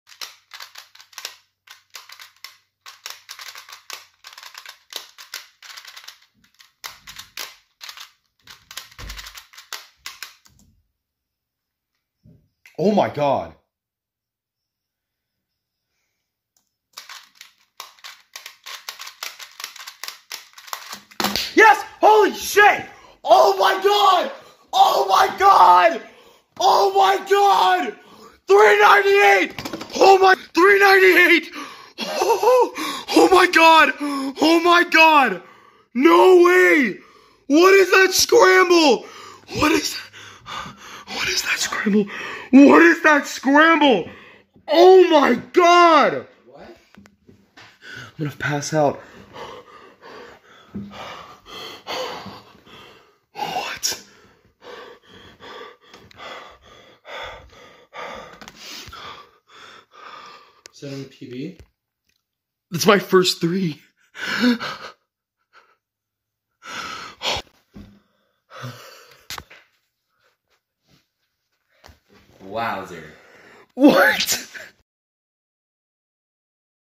3.98 Clock PB single!!!
WHAT! First sub 4
8 moves wtf
Cross Skip!

Scramble: UR1+ DR4+ DL1+ UL2- U1+ R5- D5- L1+ ALL6+ y2 U2- R0+ D2- L6+ ALL0+ UR DR UL

8 moves/3.98= 2.01 TPS!!!

I would never have expected to get this time so soon

I literally average 7 so getting this is absolutely insane